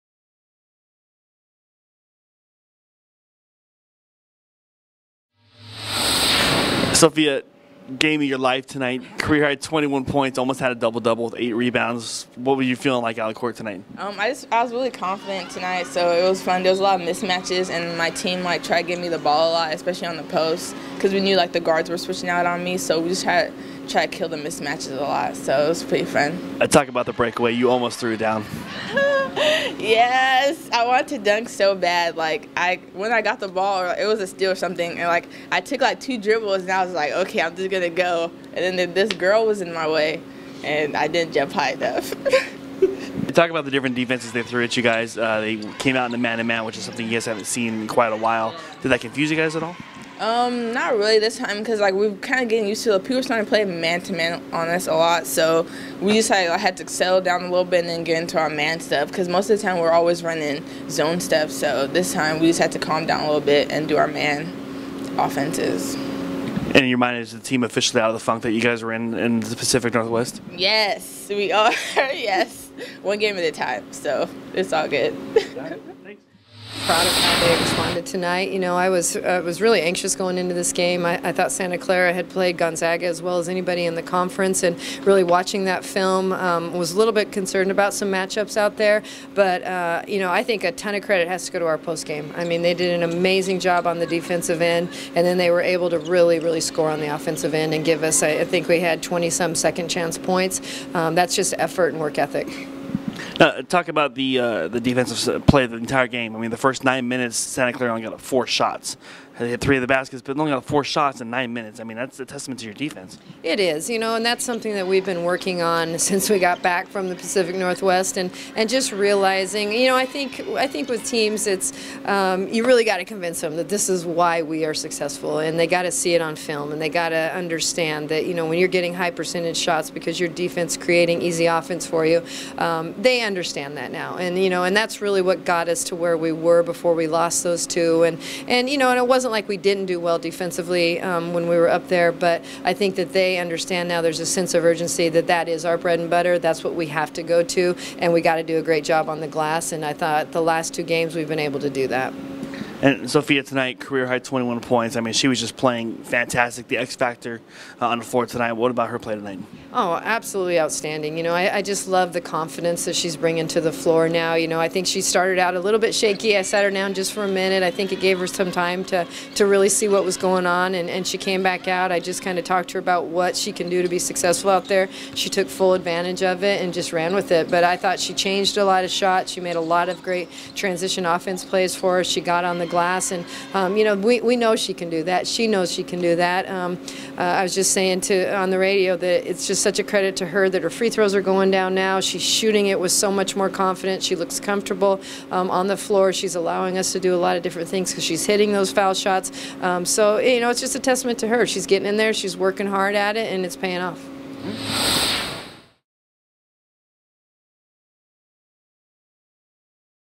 USD WBB Postgame Interview Against Santa Clara
Check out the University of San Diego women's basketball team's postgame interview after a 79-55 win over Santa Clara to continue their home game winning streak.